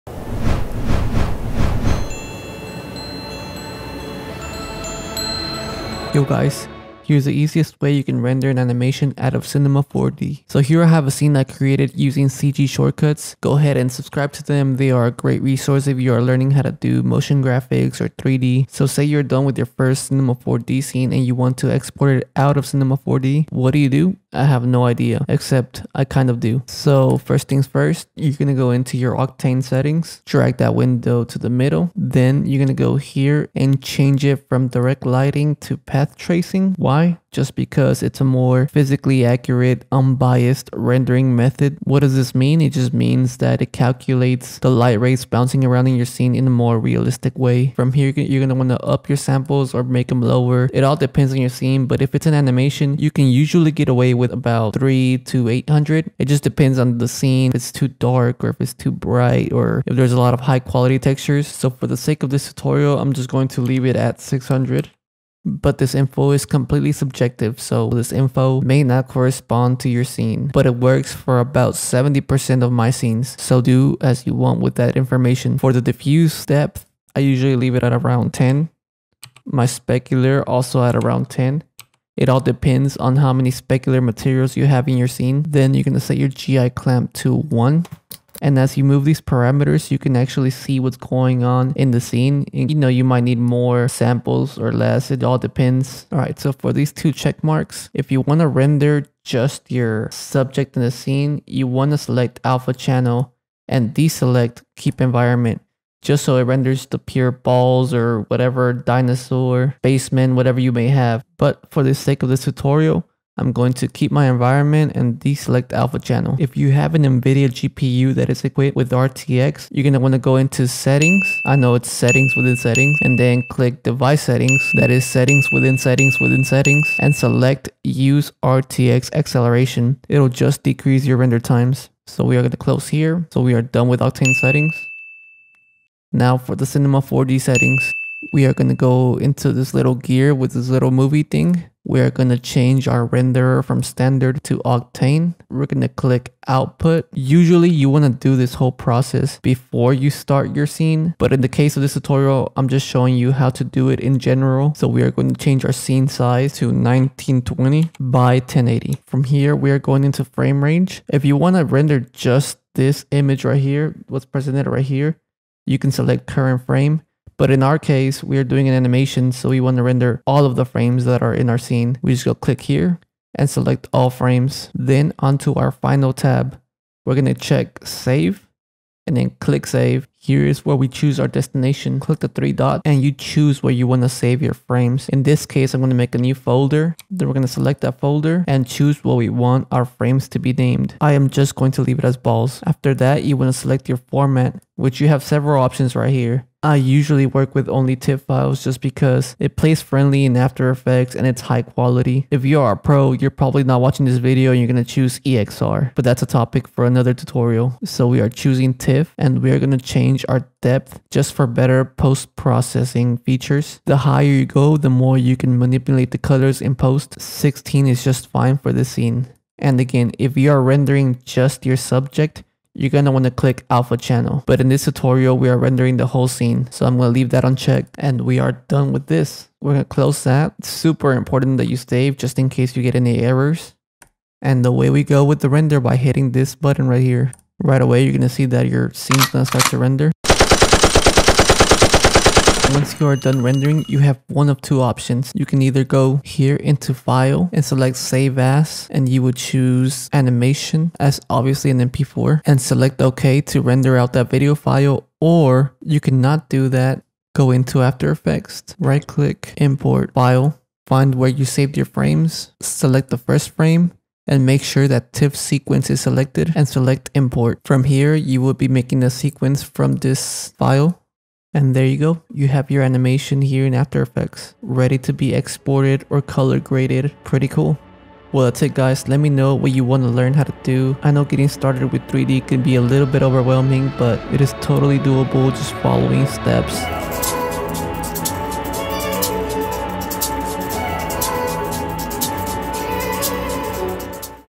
Cinema 4D & Octane Tutorial - How to Export an Animation
In this tutorial I'll show you my go-to settings for rendering out an animation sequence in C4D using Octane.